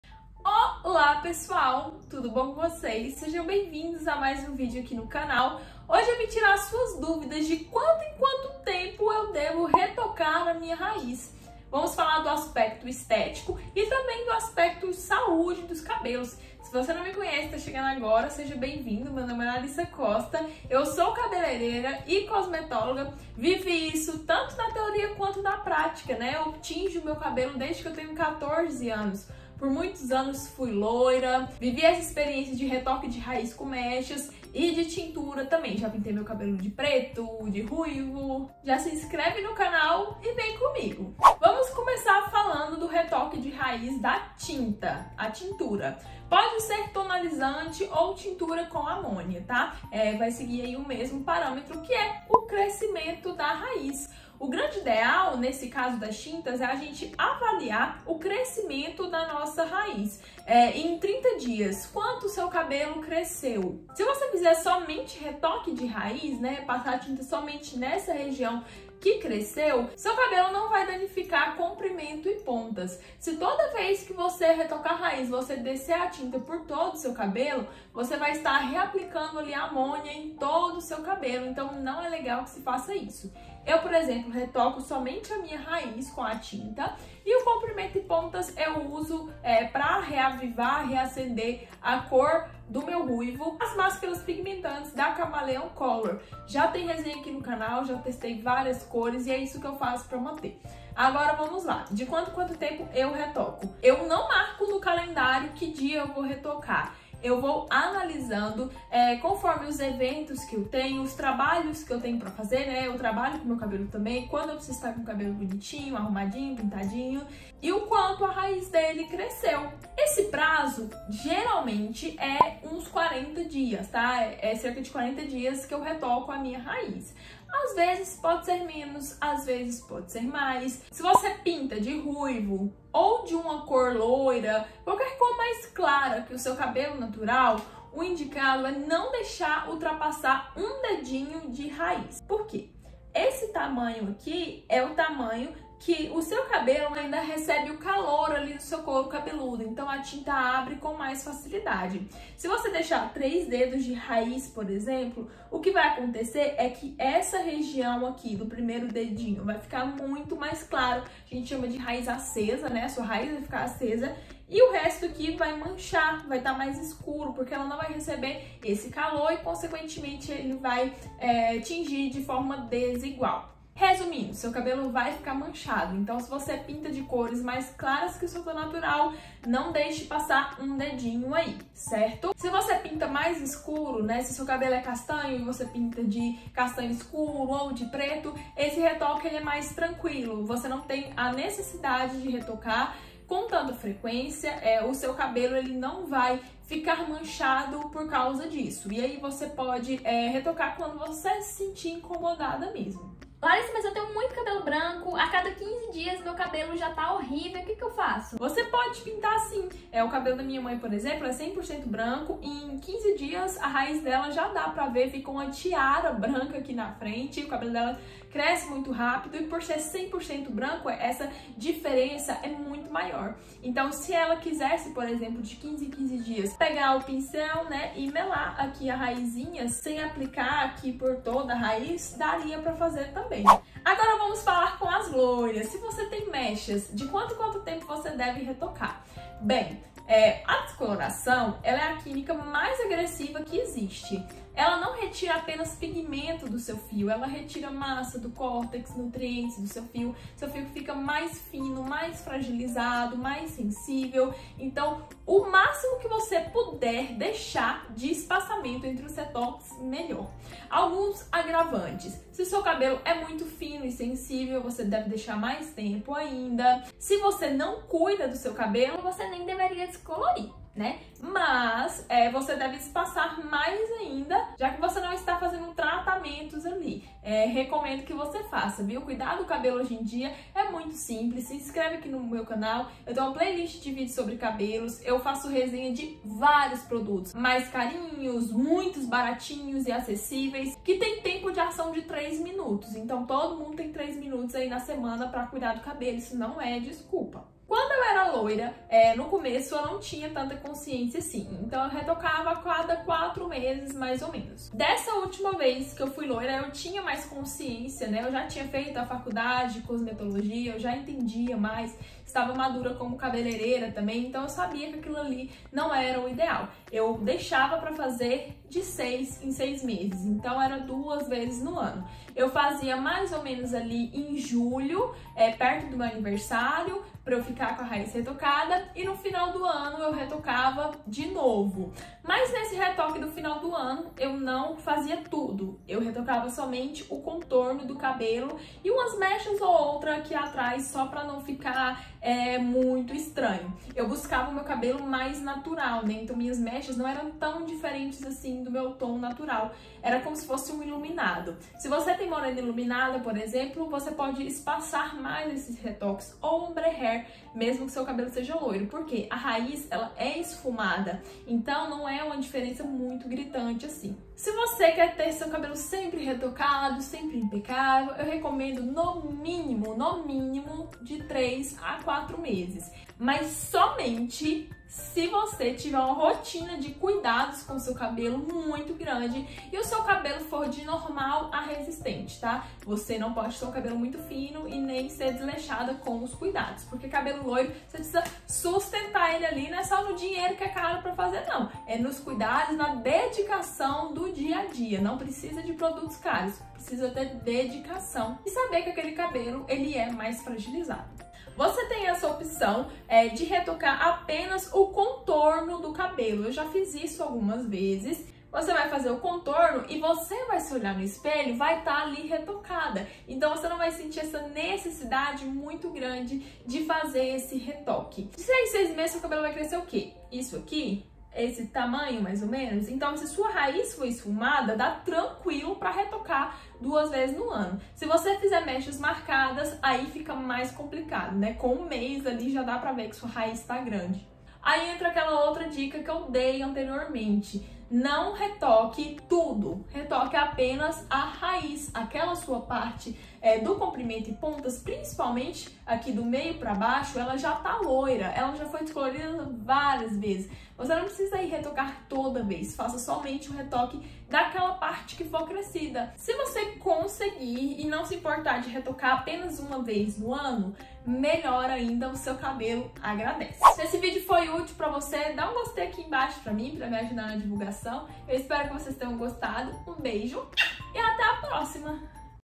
De quanto em quanto tempo devo RETOCAR A RAIZ
Com qual frequência você retocar a raiz?
Essa é uma dúvida muito frequente entre as pessoas que pintam ou descolorem o cabelo, por isso fiz esse vídeo, para você entender quando deve retocar sua raiz, qual frequência é saudável para o cabelo e ideal esteticamente, afinal, uma raiz muito crescida não é muito bonita e afeta a nossa auto estima.

Sabendo de quanto em quanto tempo você deve retocar a raiz, você pode analisar e se programar para estar sempre com o retoque de raiz em dias